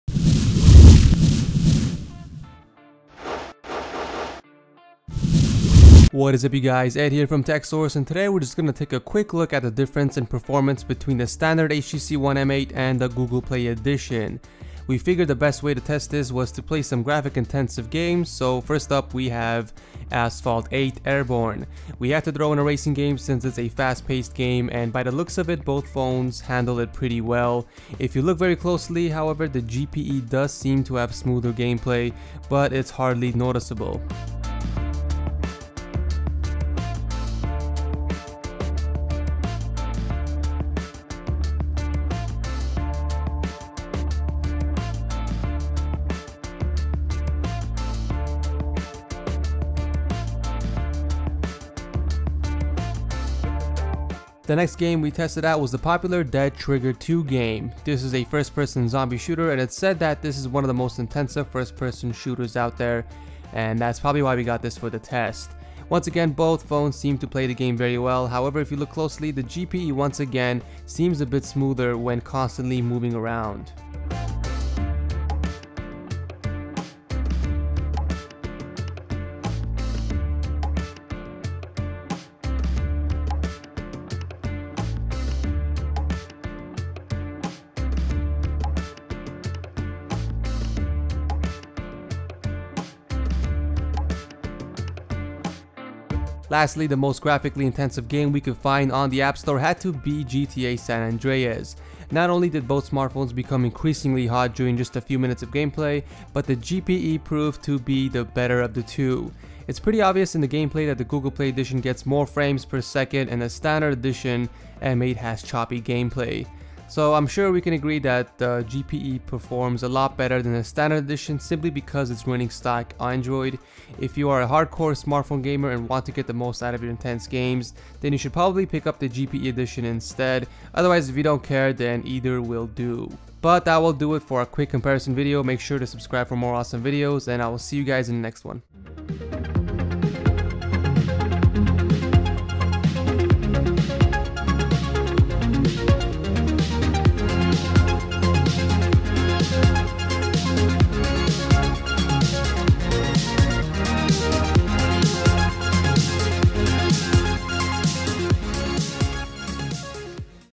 HTC ONE M8 GPE (Google Play Edition) vs Standard - Performance Comparison
Comparing the Performance of the HTC ONE m8 GPE (Google Play Edition) to the HTC ONE M8 Standard edition.

To get the most accurate results we compared them using graphic intensive games seeing as it uses the most resources on both devices.